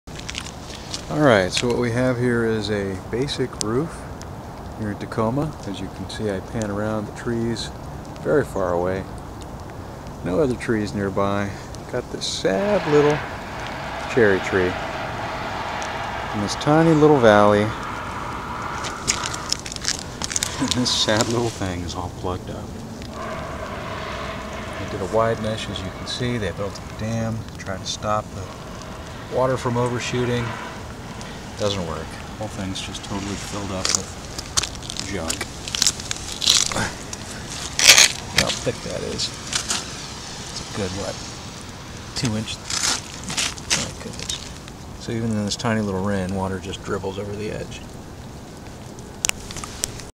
Leaffilter gutter guard in Tacoma, WA
Best possible scenario for a gutter guard - NO TREES. Still failed!

Find a system that works with pine and fir needles 9and everything else)